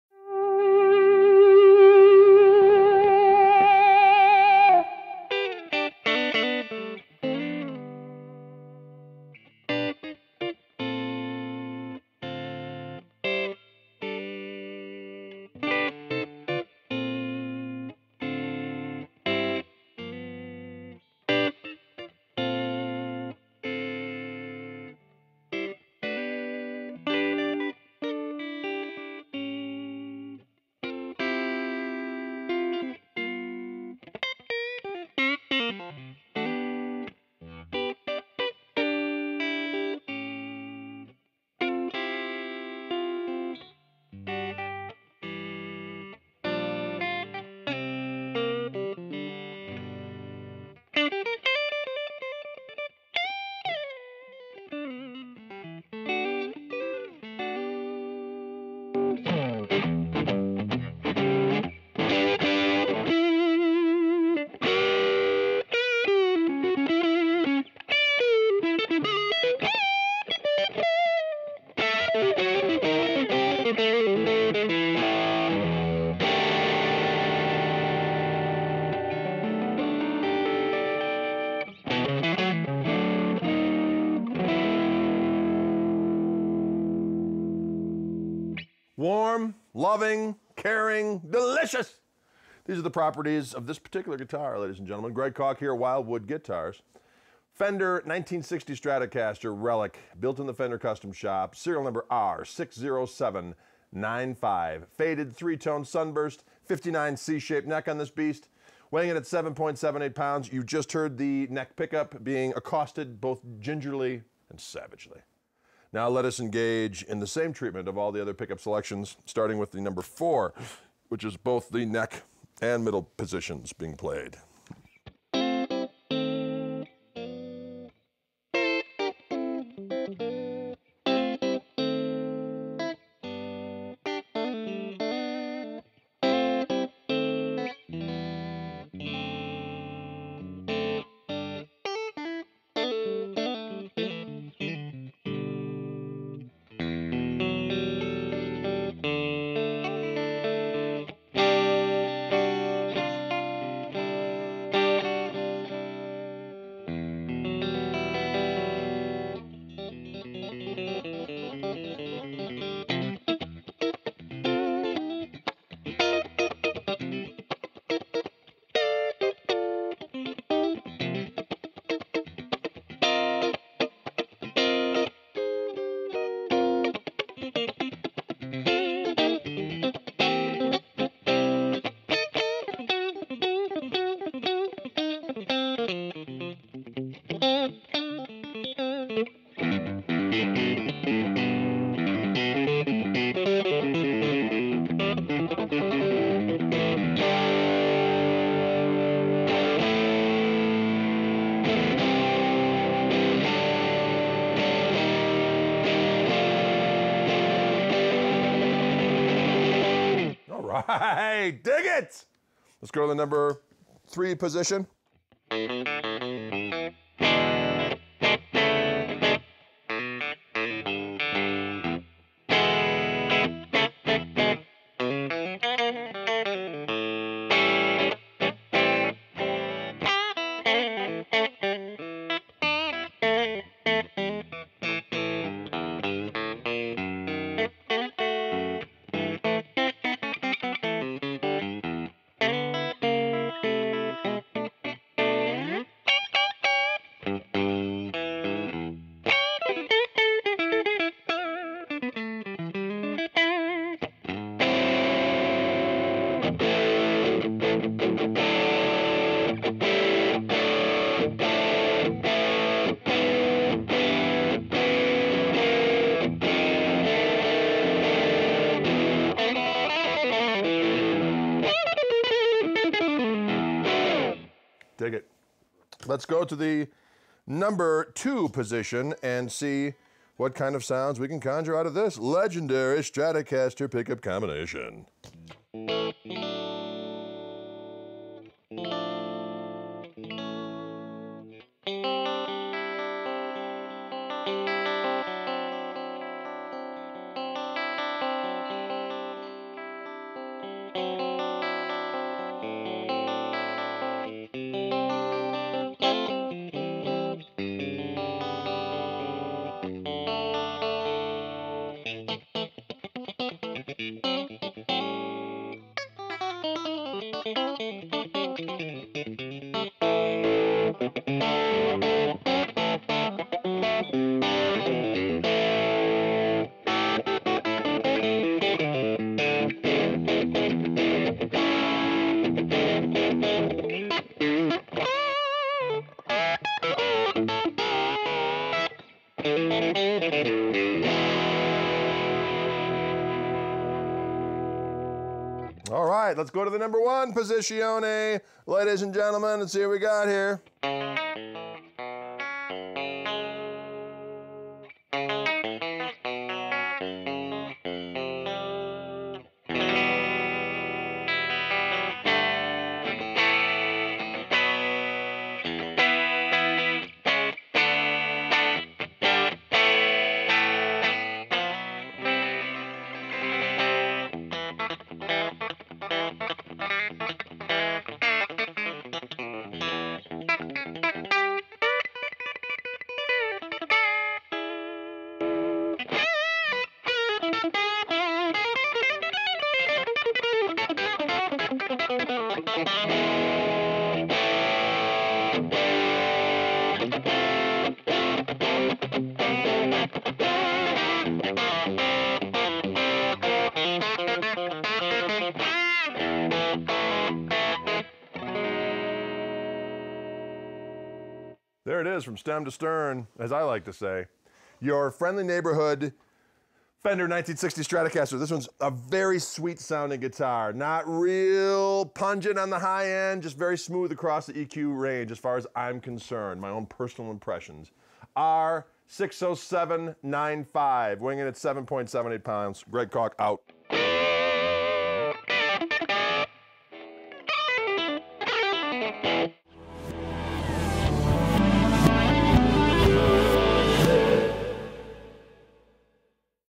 Fender Custom Shop 1960 Stratocaster Relic • SN: R60795
At the dawn of the 1960s, with manned space flight just beginning, the Stratocaster stood poised for a wild new decade full of exciting and innovative new music. Music history beckoned as never before-sound would be wrung from the Stratocaster in the '60s that its designers and players of the previous decade couldn't possibly have dreamed up. The Fender Custom Shop 1960 Stratocaster Relic Electric Guitar features an alder body with lacquer finish, maple neck, rosewood fingerboard, 9.5"" radius, 6105 frets, 1960 Stratocaster pickups with 5-way switching, custom pickguard and relic vintage hardware. This detailed Custom Shop reproduction of a 1960 Strat was built utilizing original tooling and production techniques of the era. And its Relic finish treatment gives it the look of a well-traveled and heavily played axe, although it is absolutely new in setup and playability. Includes deluxe hardshell case.

Brand: Fender Custom Shop
Model: '60 Stratocaster Relic
Type: Stratocaster
Year: 1960
Finish Color: Faded 3-Tone Sunburst
Aging: Relic
Finish Type: Nitrocellulose Lacquer
Weight: 7.78 lbs.
Body Wood: Select Alder
Neck Wood: Maple
Neck Shape: '59 Large "C"
Fingerboard: Rosewood
Fingerboard Radius: Custom 9.5"
Scale Length: 25.5" (648 mm)
Width at Nut: 1.650" (42 mm)
Frets: 21 Dunlop 6105
Pickups: 3 Abigail Handwound Single-Coils
Controls: Master Volume, 2 Tone, 5-Way Switch
Hardware : Nickel/Chrome
Bridge: American Vintage Synchronized Tremolo
Tuners: Fender/Gotoh Vintage Style
Pickguard: 3-Ply White
Case: Deluxe Brown Hardshell Case (Gold Interior)
Case Candy: Strap, Cable
COA: Yes